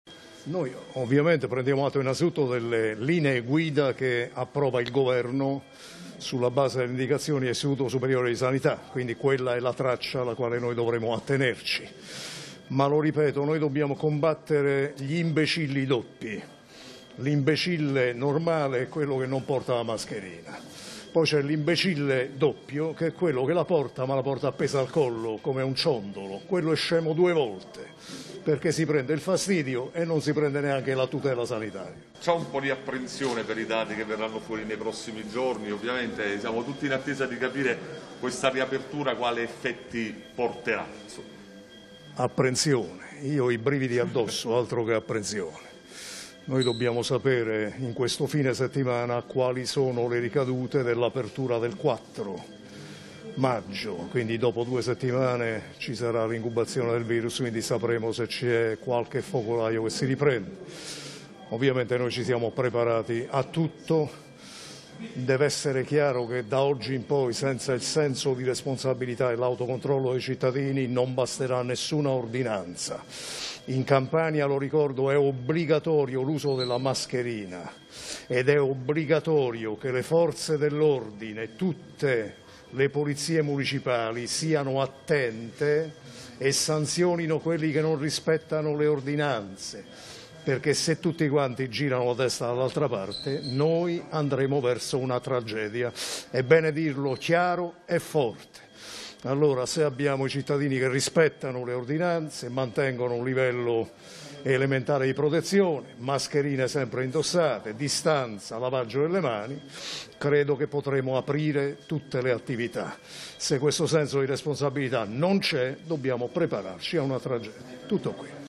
Fase 2, De Luca: «Chi porta la mascherina al collo è un imbecille doppio»
Il governatore della Regione Campania ammette di avere paura sui dati dei prossimi giorni

«Dobbiamo combattere gli imbecilli doppi: l'imbecille normale è quello che non porta la mascherina; poi c'è l'imbecille doppio, che è quello che la porta ma la porta appesa al collo, come un ciondolo Quello è scemo due volte, perché si prende il fastidio di portare la mascherina e non si prende neanche la tutela sanitaria». Così il presidente della Regione Campania Vincenzo De Luca a margine dell'inaugurazione del nuovo reparto di terapia intensiva da 12 posti al Covid hospital di Boscotrecase (Napoli). Sui dati dei prossimi giorni relativi al contagio De Luca dice: «Apprensione? Io ho i brividi addosso. Noi dobbiamo sapere in questo fine settimana quali sono le ricadute dell'apertura del 4 maggio». ( Ansa - CorriereTv ). Guarda il video su Corriere